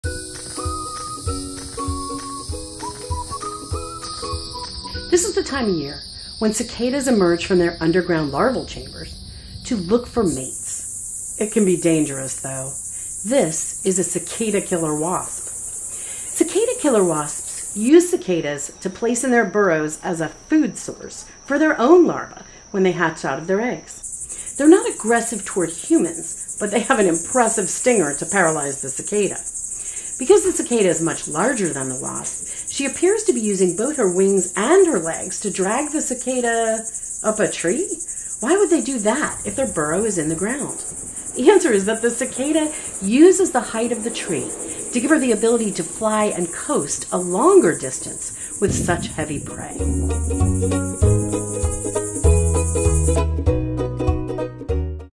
Cicada Killer Wasp
It’s the time of year when cicadas emerge from their underground larval chambers to look for mates.  It can be dangerous though.  This is a Cicada Killer Wasp.  Cicada killer wasps use cicadas to place in their burrows as a food source for their own larvae, who hatch out of eggs.  They are not aggressive toward humans, but they have an impressive stinger to paralyze the cicada. Because the cicada is much larger than the wasp, she appears to be using both her wings and legs to drag the cicada… up a tree?  Why would they do that if the burrow is in the ground?  The cicada will use the height of the tree to give her the ability to fly a longer distance with such heavy prey!